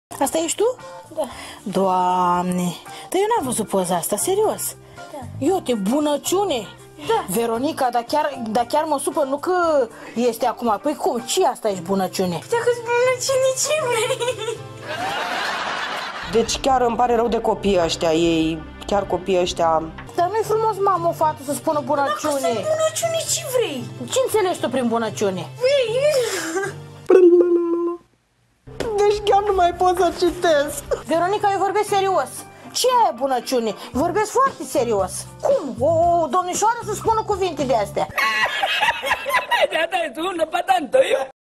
Daca-s bunaciune ce vrei (seria ”NU APA”) Ep. 2
Atunci cand te uiti cu mama ta pe profilul de Facebook
Din seria ”Schimb de mame” ( NU APA ) Episodul 2